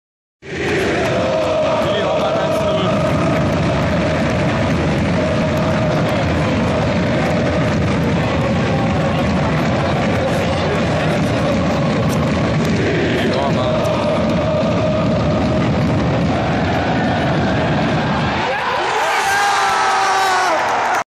1. FCK vs. Erzgebirge Aue 1:0 mit Ansage
Willi Orban mit dem 1:0 gegen den FC Erzgebirge Aue